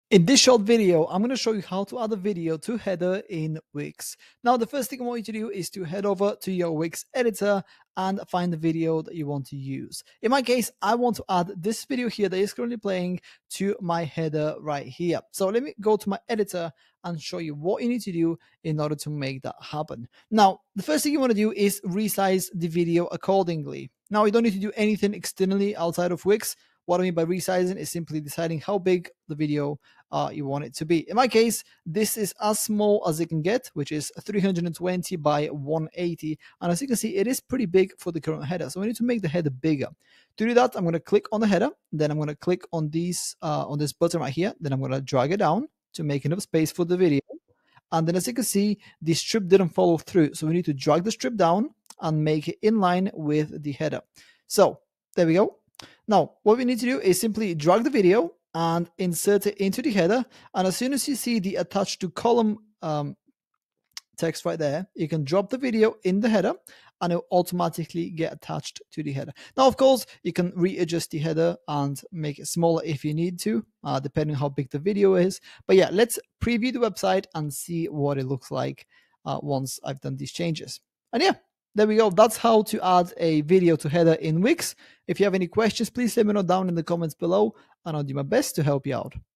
How to Add a Video to Header in Wix [QUICK GUIDE]
Let's take a look at how to add video to header in Wix.